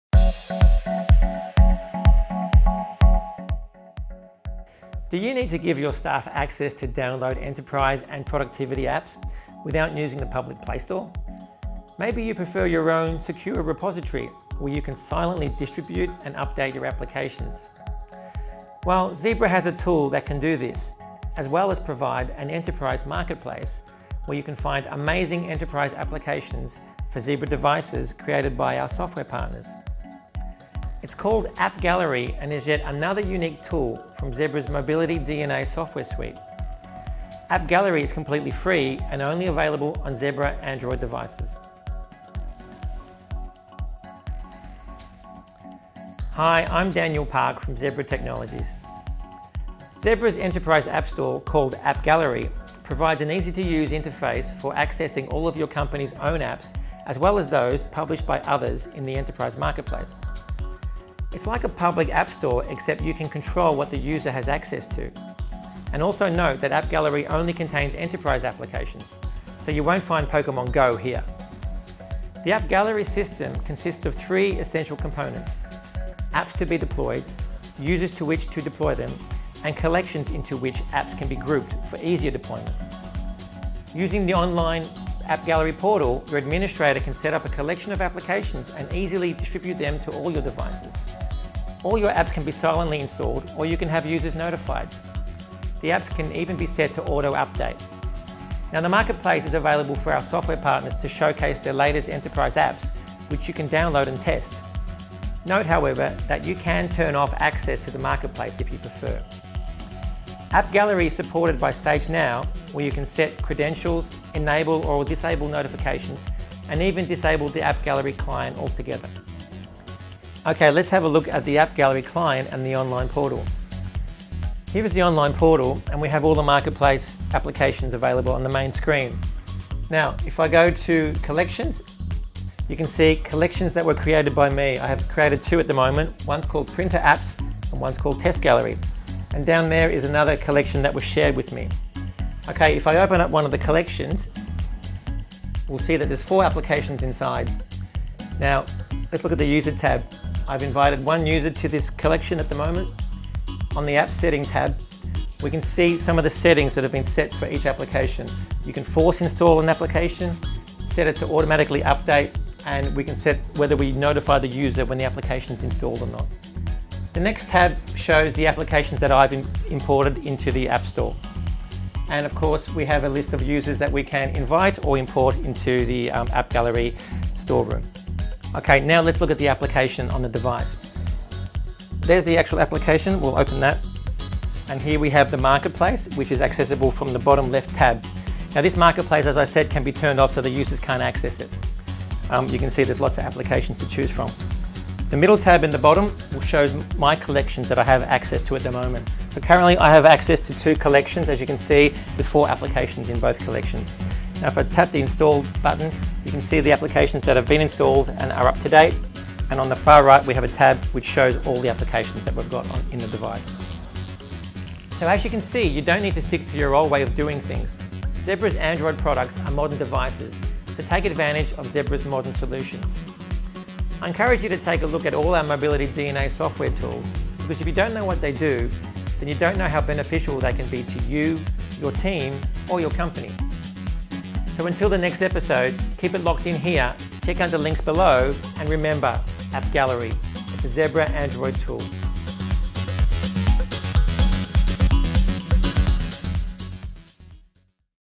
Zebra Mobility DNA: It’s a Zebra Android™ Tool – AppGallery
In this video, Zebra demonstrates how to discover, purchase, deploy and update enterprise apps that make the most of Zebra mobile computers.